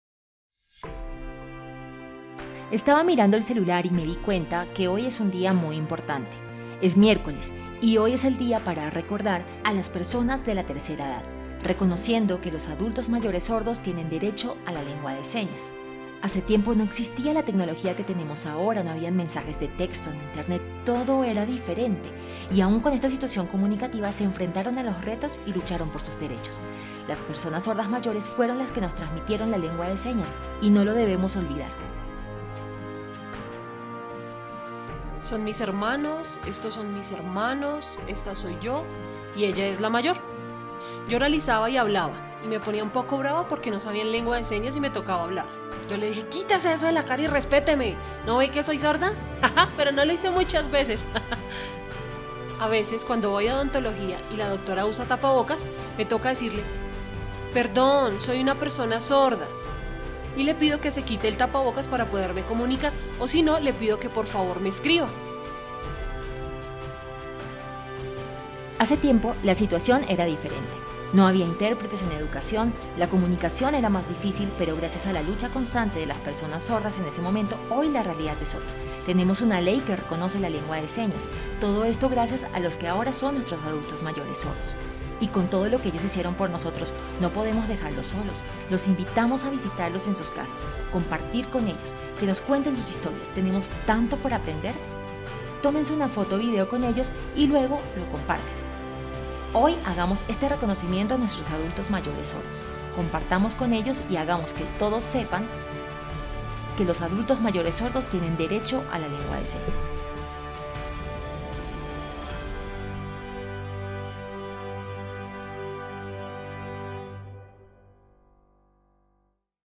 Día del Sordo Adulto Mayor
Fueron ellos quienes abrieron el camino a la lucha por el respeto a la lengua de señas. Sin sus enseñanzas no habríamos logrado lo que hemos conseguido hasta hoy. Nos toca a los jóvenes de ahora continuar CONSTRUYENDO EL FUTURO DE NUESTROS NIÑOS.